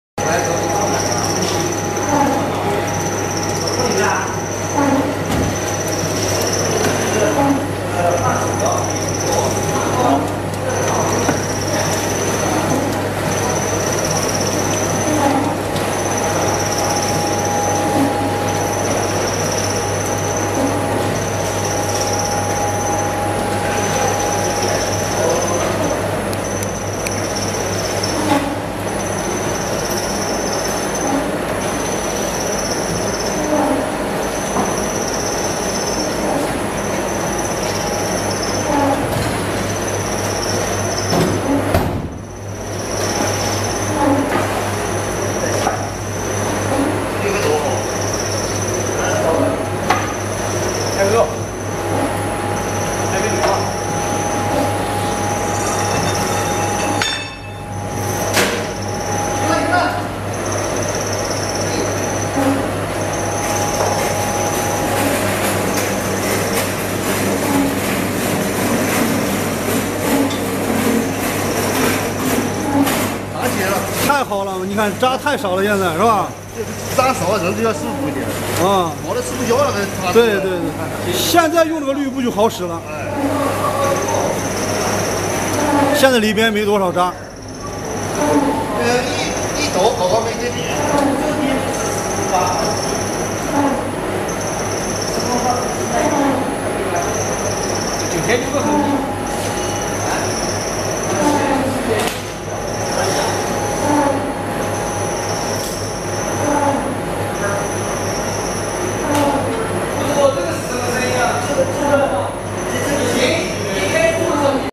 Automatic Cold Oil Screw Press Machine Oil Production Shipped By Sea 启亿
This video expresses an oil production process using automatic technology. Zhengzhou QI'E Grain and Oil Machinery Co., LTD is specialized in producing grain and oil machines, operating  pretreatment, extraction, and refinery.